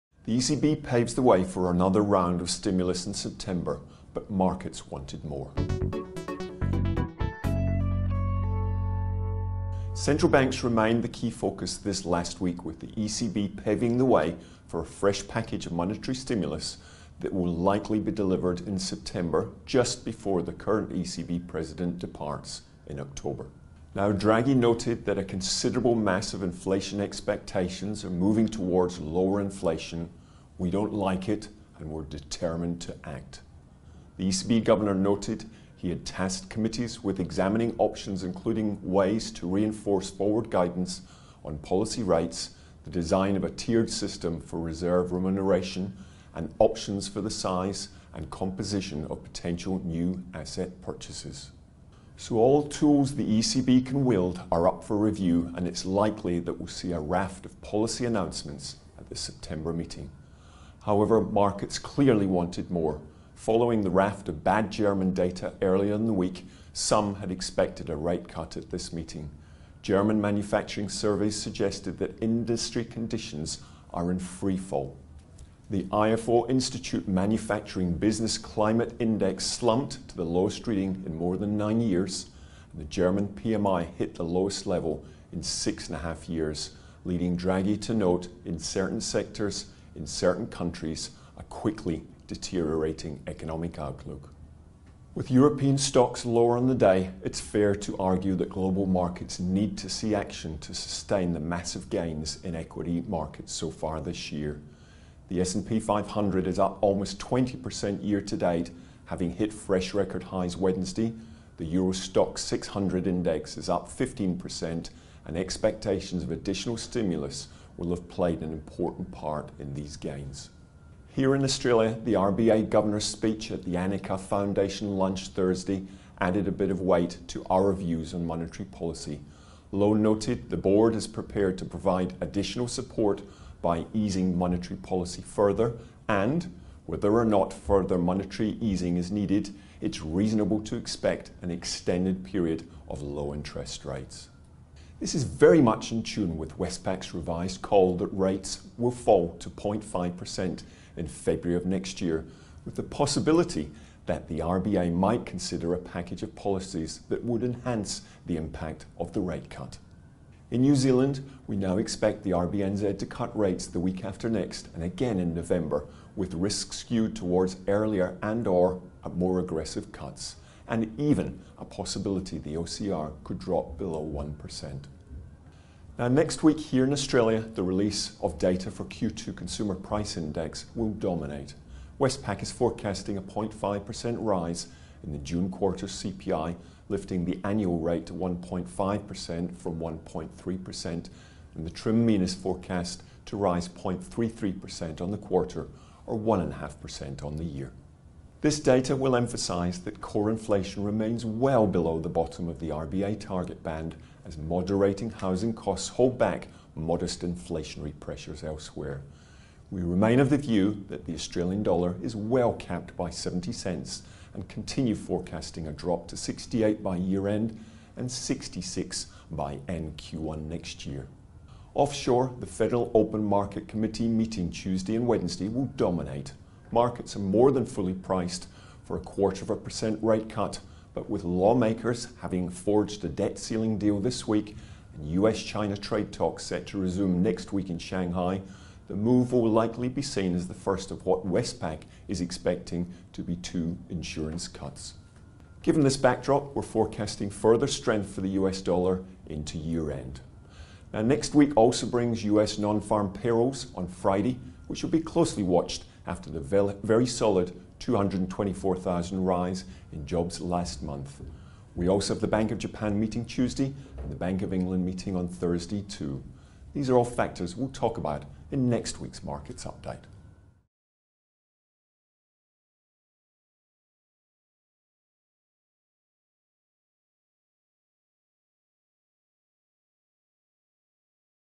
Westpac Markets Update Rob Rennie July 26, 2019
Central banks remained the key focus this last week with the ECB paving the way for a fresh package of monetary stimulus that will likely be delivered in September, just before the current ECB President departs in October.